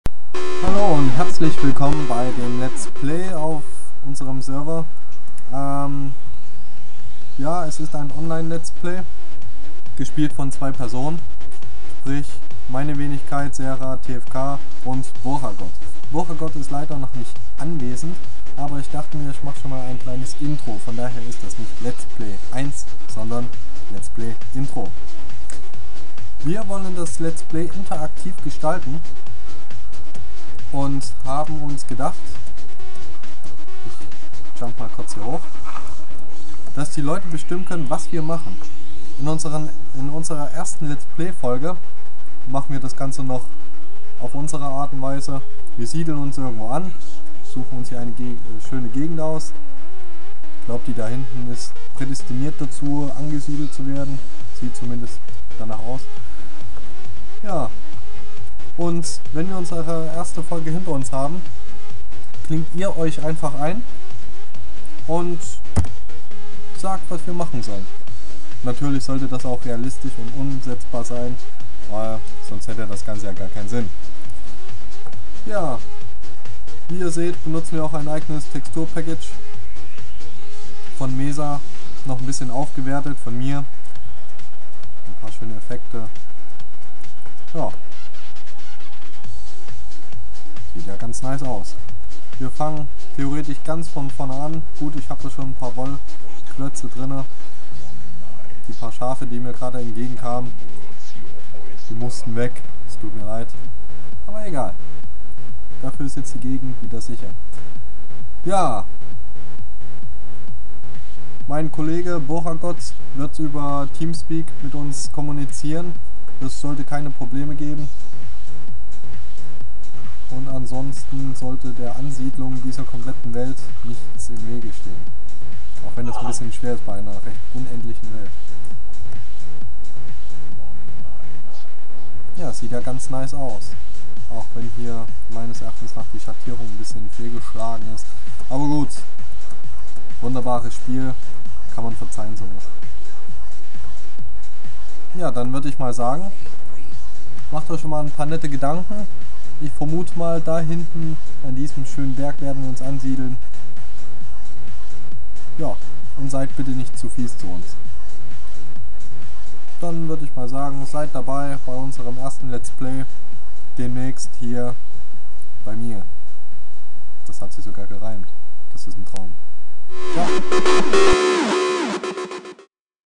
LetsPlay-Intro
Einleitung in unsere LetsPlay Reihe.
Ihr könnt euch Interaktiv mit an diesem LetsPlay beteiligen.
Begleitet wird es von Boragod und ZeraTFK.